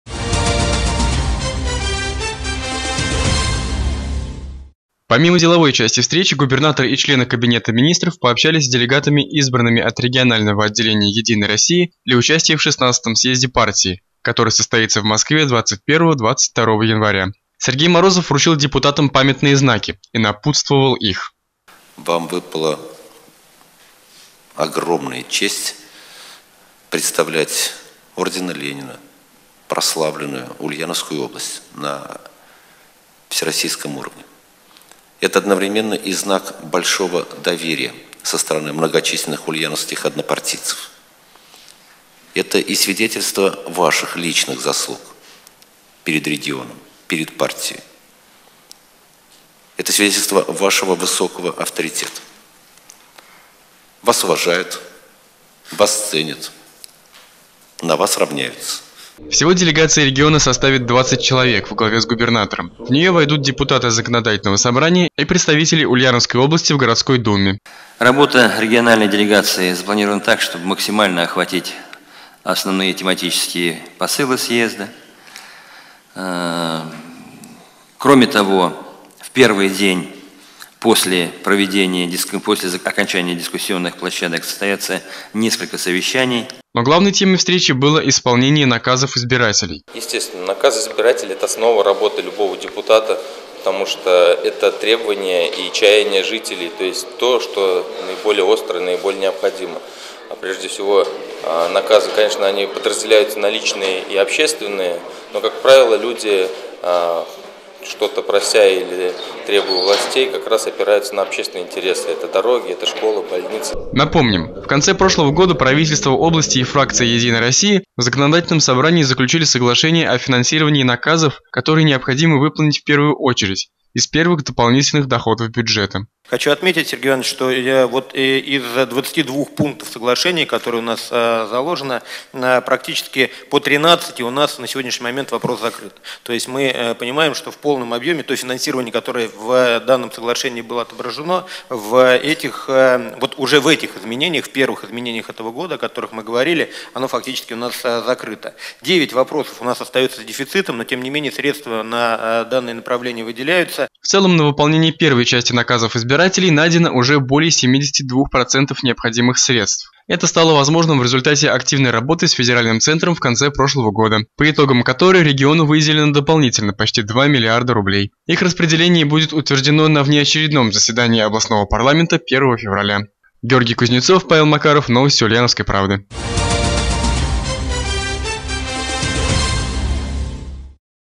Сергей Морозов встретился с фракцией «Единой России» ЗСО
Правительство Ульяновской области распределило 2 млрд. 16 млн. рублей дополнительных доходов, 1,443 млрд. из которых пойдут на исполнение предвыборных наказов избирателей.